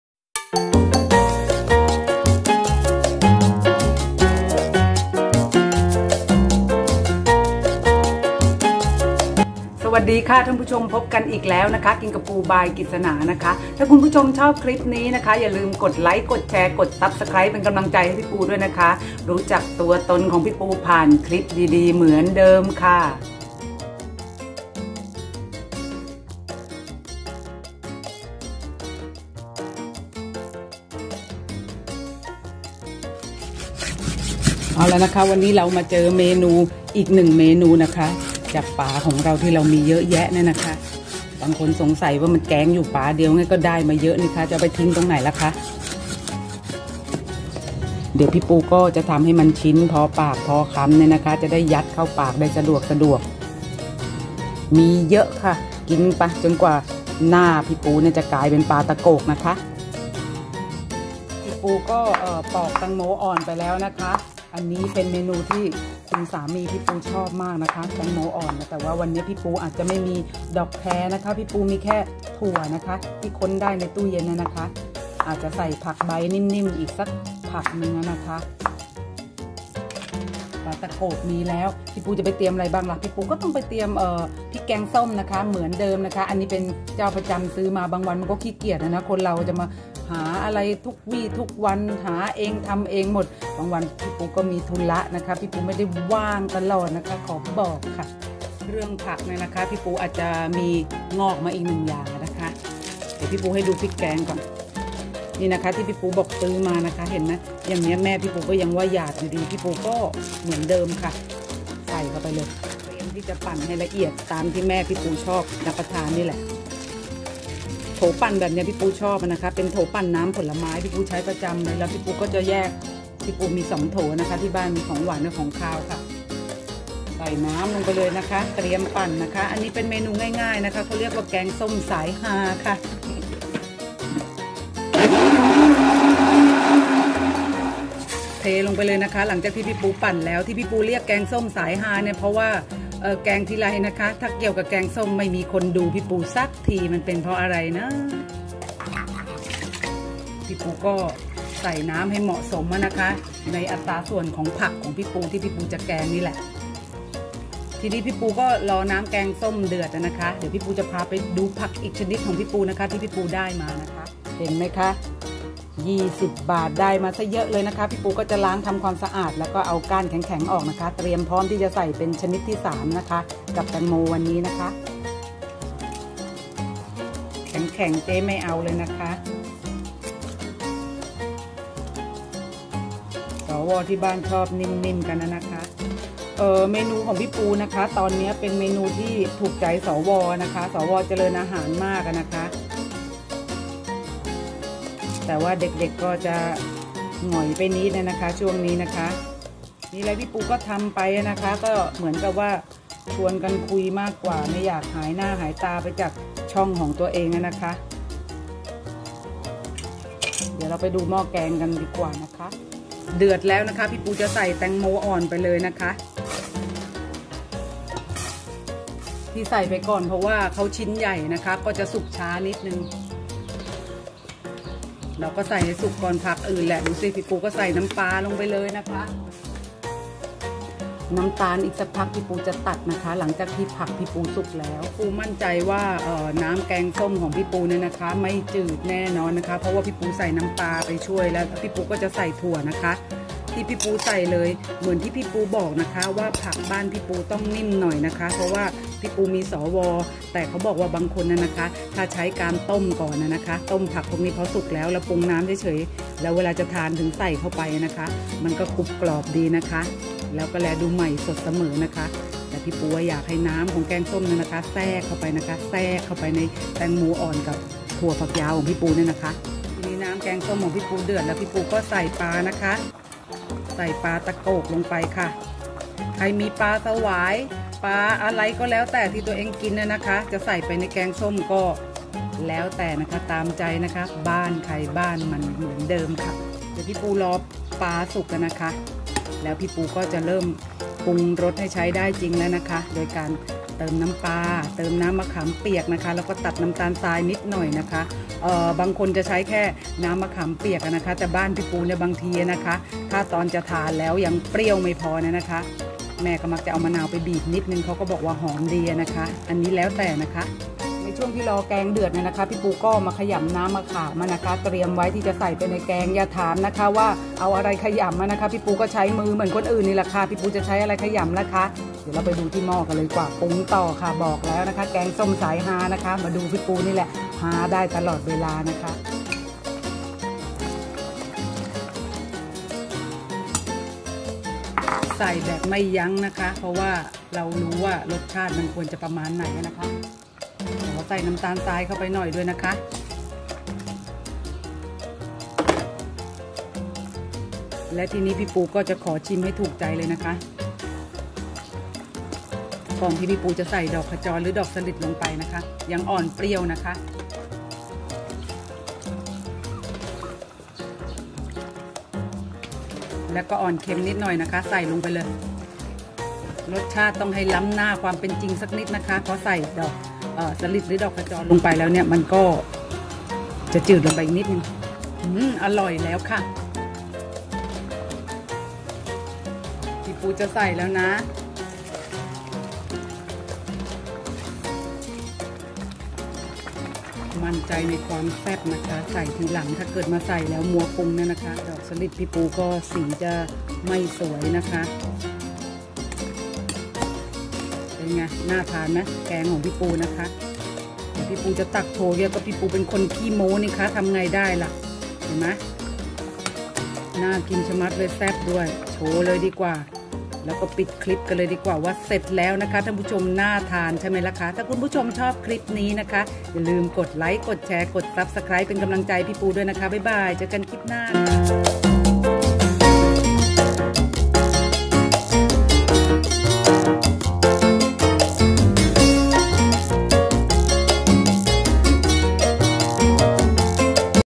แกงส้มดอกขจรใส่ปลาตะโกก#กินกับปู
ช่วงนี้จะชวนกินเมนูบ้านๆบ่อยหน่อยนะคะ
สว.เรียกร้องค่ะลองมาดูแกงส้มสายฮาพี่ปูกันเลยค่ะ